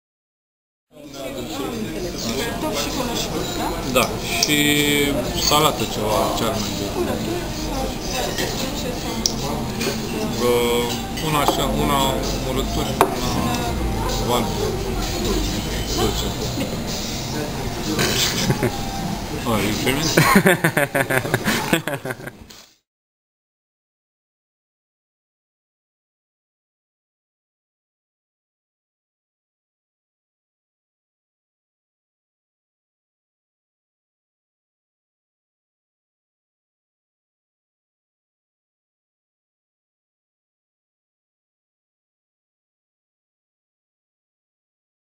Random fun times in Romania
Had a short time but great time in Romania; here's a little of it...